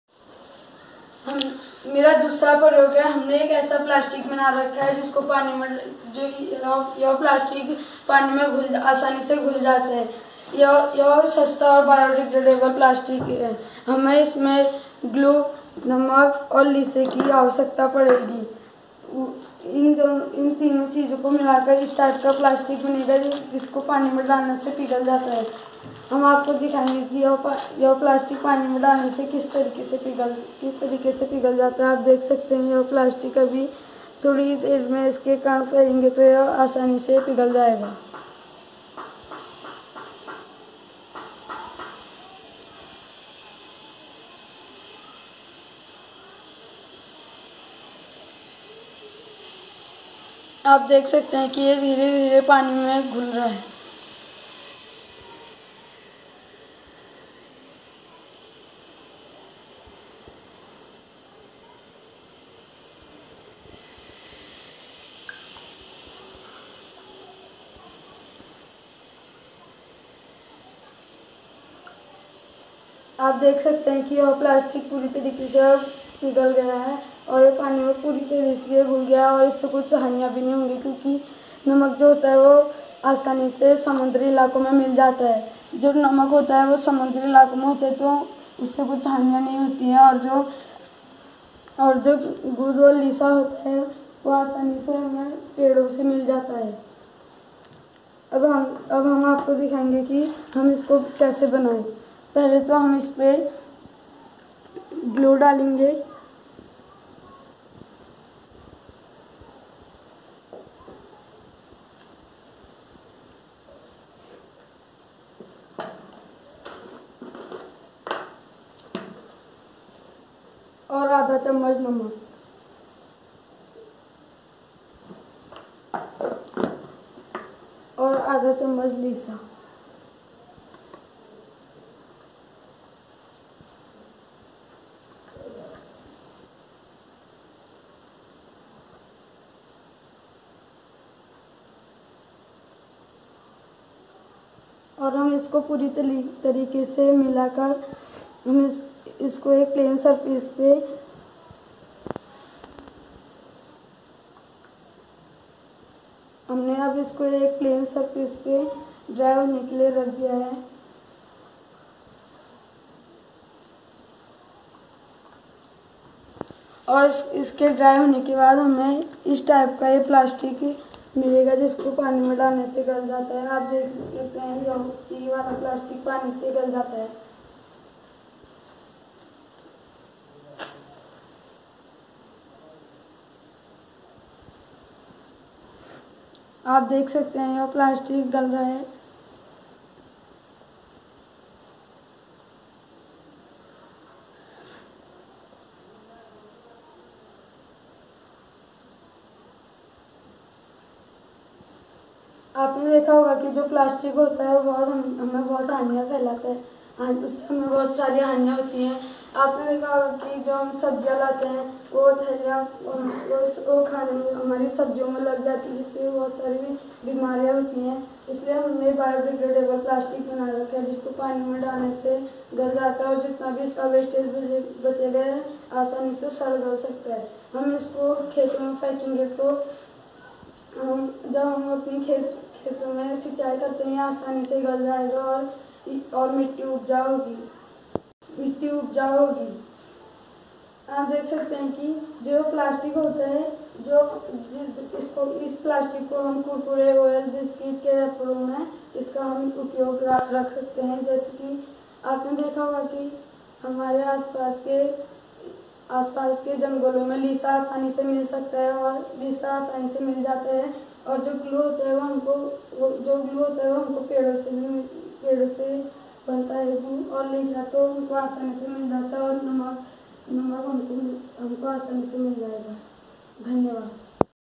बायोडिग्रेडेबल प्‍लास्‍टिक (लीसाथीन), बाढ़ अलार्म
This exhibit demonstrates the preparation of biodegradable plastic by using salt, glue, leesha from Pine tree